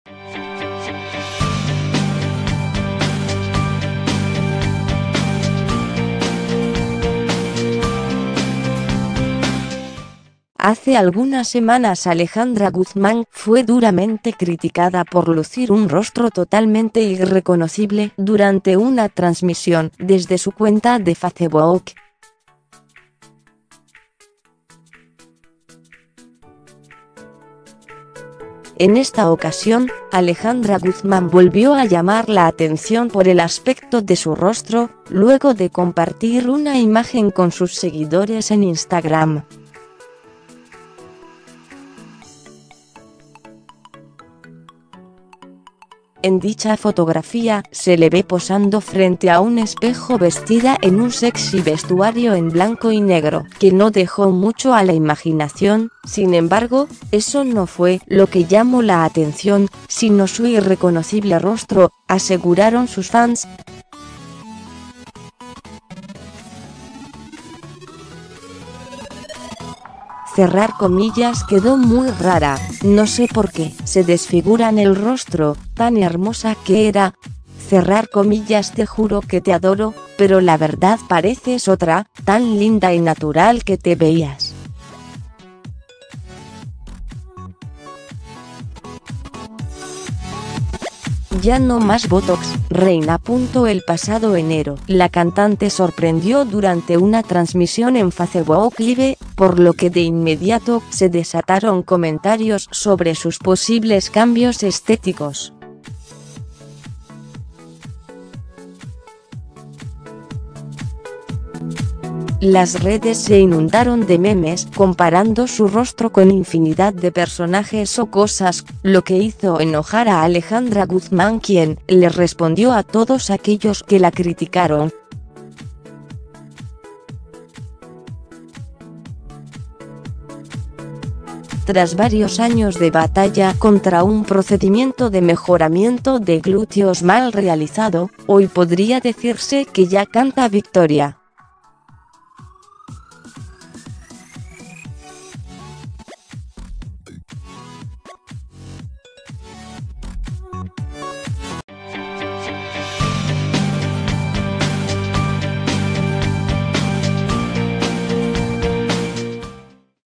Alejandra Guzmán estremece a sus seguidores ¡por nueva foto luciendo su rostro!
El rostro de Alejandra Guzmán causó controversia nuevamente por su apariencia, sus fans aseguran que se veía mucho mejor antes de ‘tanto bótox’.

Fuente: goo.gl/2LdGq7